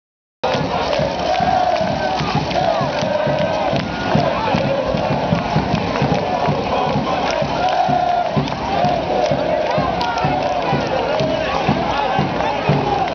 Handball GWD-Minden
GWD Minden schlägt THW Kiel, Humba!!!